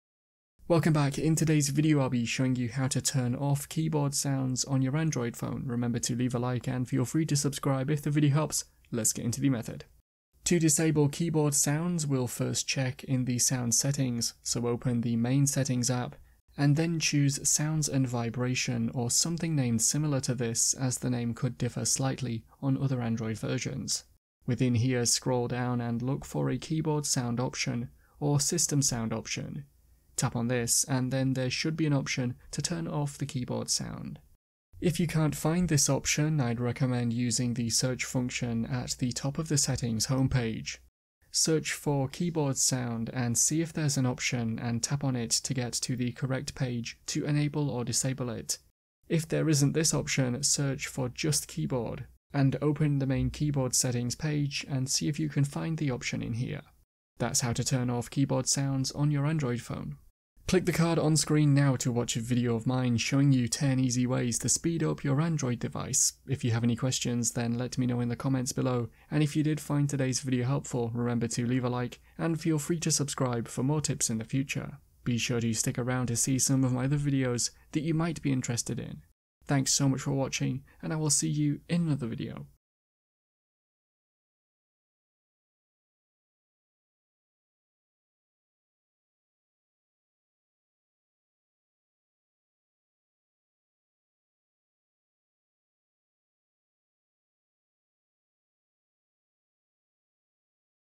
How to Turn OFF Keyboard Sounds on Android Phone
Want to know how to turn off keyboard sounds on Android phone? This video will show you how to disable keyboard sounds on Android phone. You might want to know how to turn off keyboard sound on Android phone if you would rather have a silent keyboard without the keyboard clicks. Hopefully today’s video helps show you how to stop keyboard sound on Android phone!